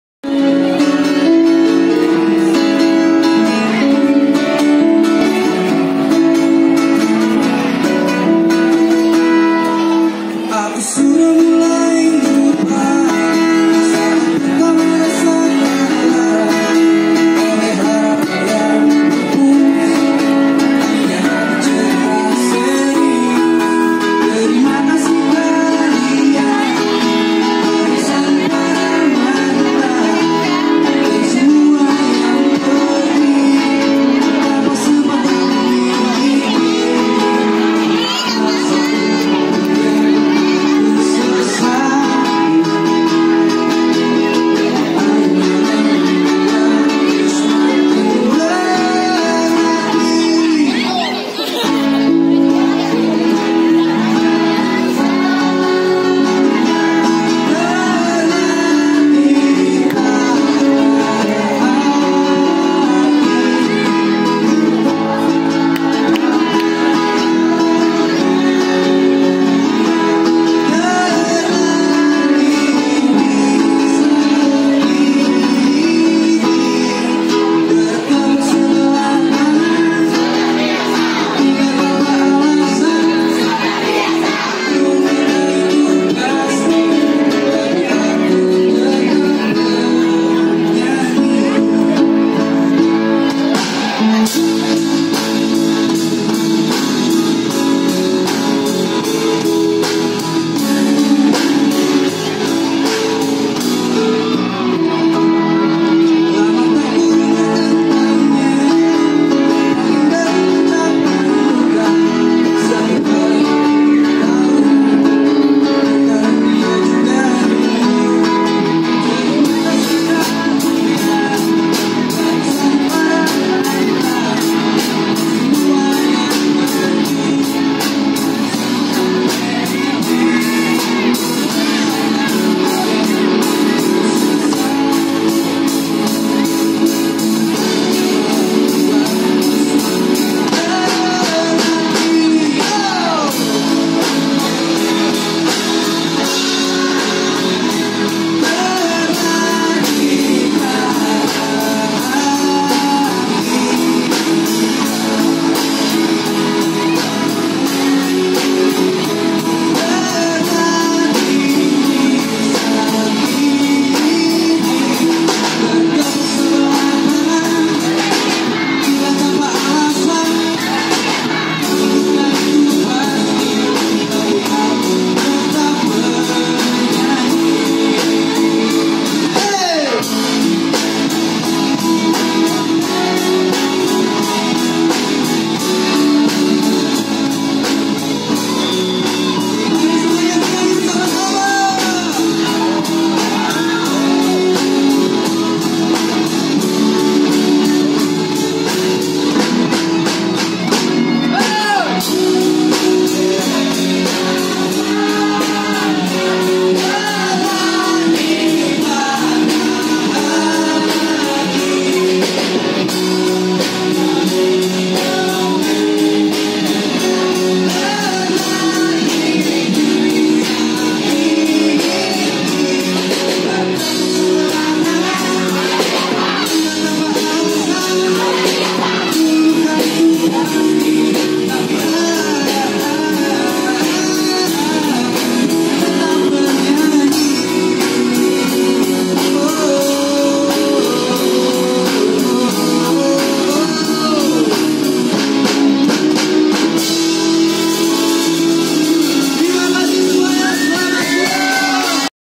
Penampilan Ekstrakurikuler Music Club SMA ISR dalam ISR CUP 2018
Penampilan Ekstrakurikuler Music Club SMA dan SMP ISR dalam gelaran acara Pentas Olahraga Seni dan Bahasa SMP ISR (ISR CUP) yang ke tiga tahun 2018.